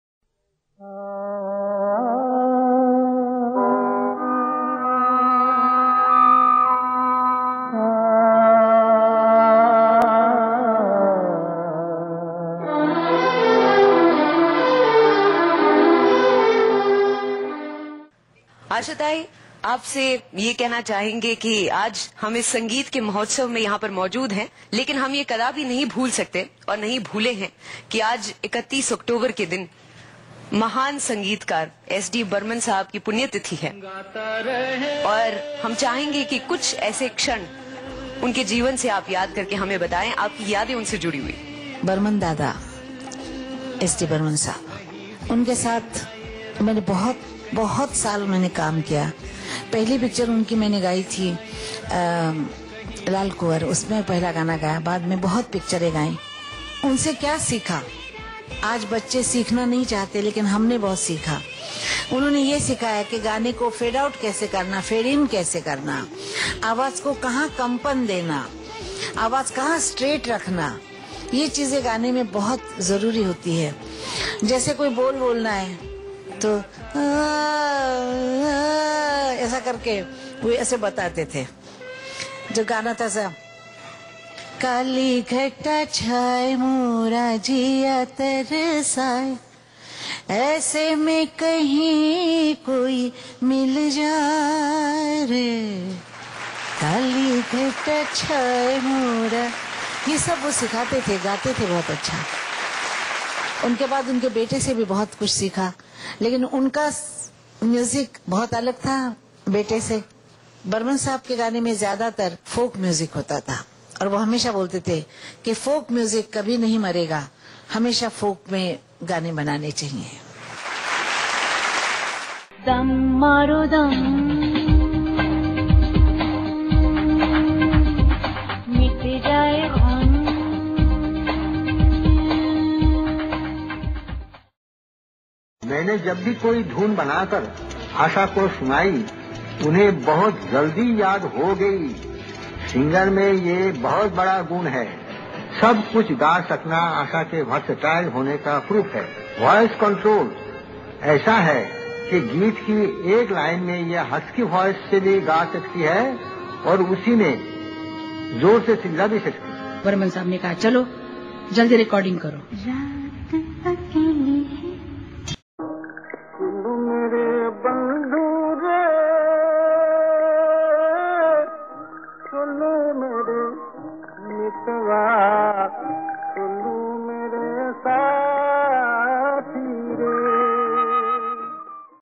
S D Burman on Asha Bhonsle And Vice Versa
S D Burman on Asha Bhonsle And Vice Versa-two legends talking about each other